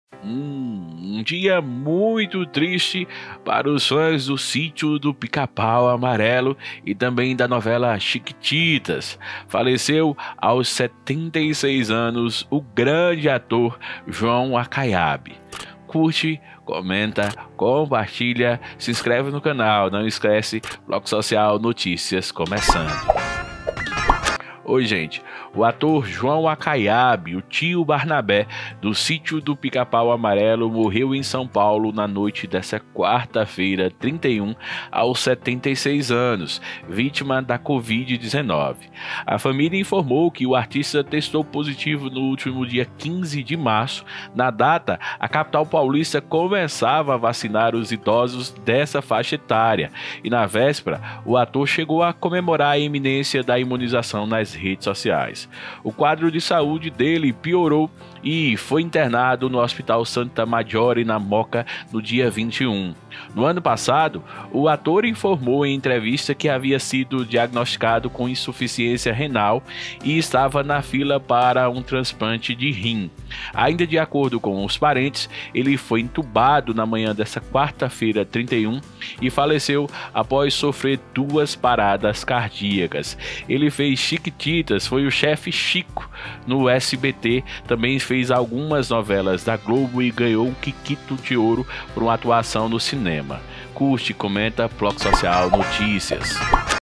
MORRE O ATOR DE CHIQUITITAS E DO SITIO DO PICA PAU AMARELO, JOÃO ACAIABE AOS 76 ANOS
O ator João Acaiabe, o Tio Barnabé do "Sítio do Picapau Amarelo", morreu em São Paulo, na noite de quarta-feira (31), aos 76 anos, vítima da Covid-19.

A família informou que o artista testou positivo no dia 15 de março.